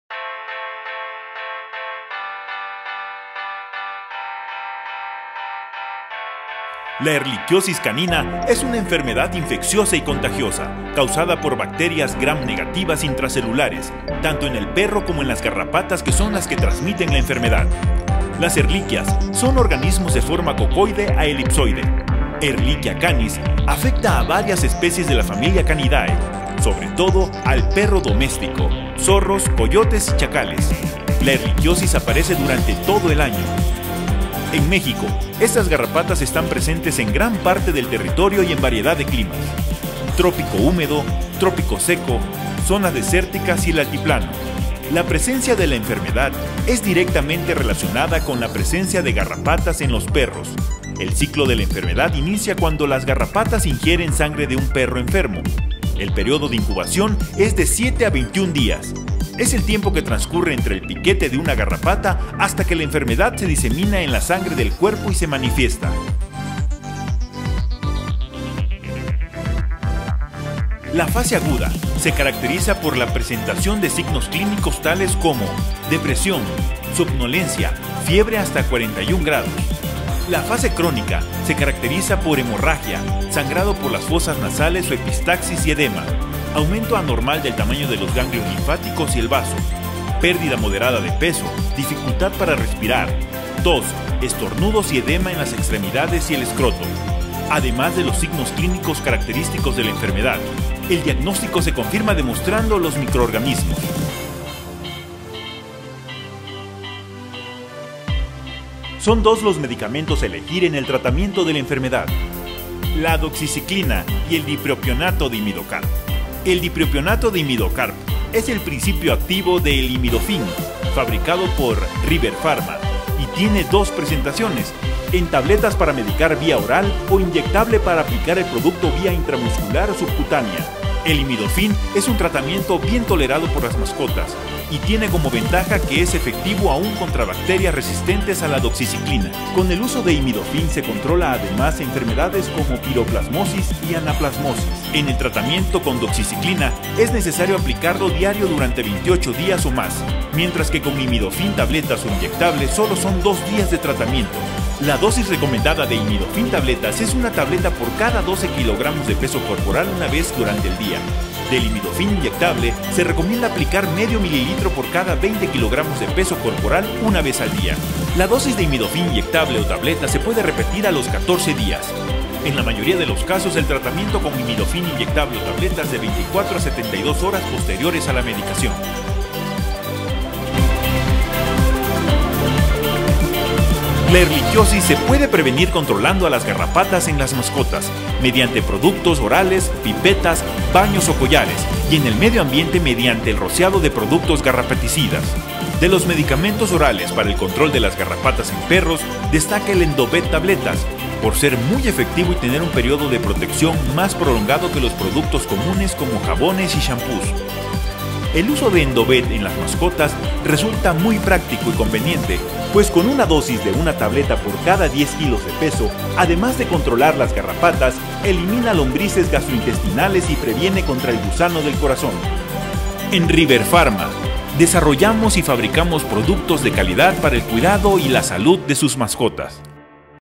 Infografía sobre la Ehrlichiosis Canina
Te compartimos nuestra infografía sobre como combatir las garrapatas en tu perro que provocan la Ehrlichiosis Canina.